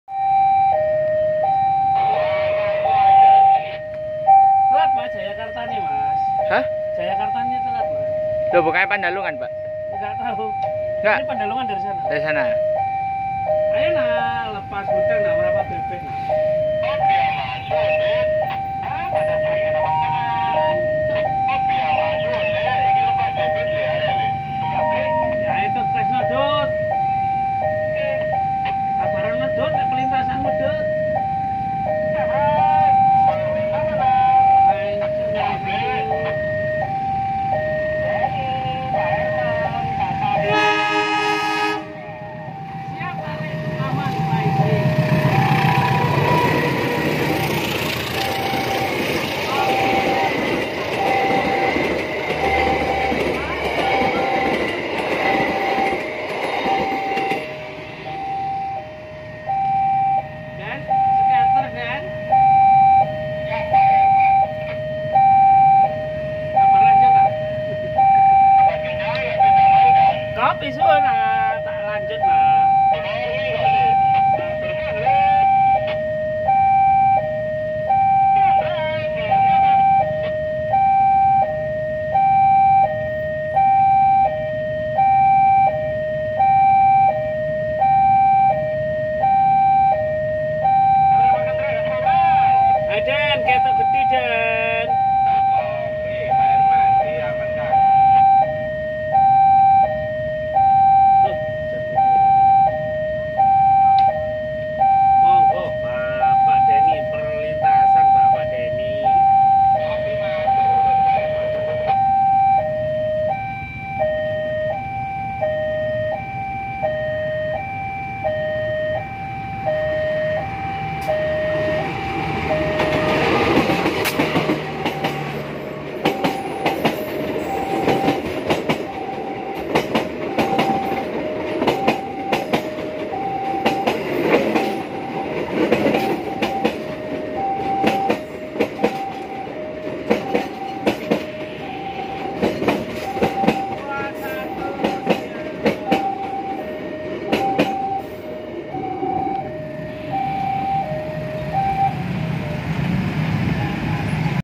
KA 430 Tumapel berpapasan dengan KA 75A Pandalungan di PJL 15 Ngagel Rejo Kidul Wonokromo Surabaya
Direkam pada tanggal 10 Oktober 2023